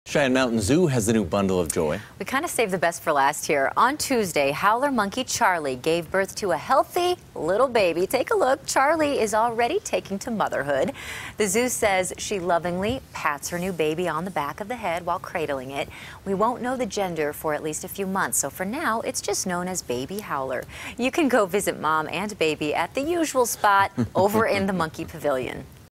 Baby Howler Monkey Born At Cheyenne Mountain Zoo
The Cheyenne Mountain Zoo in Colorado Springs welcomed a baby howler monkey on Tuesday.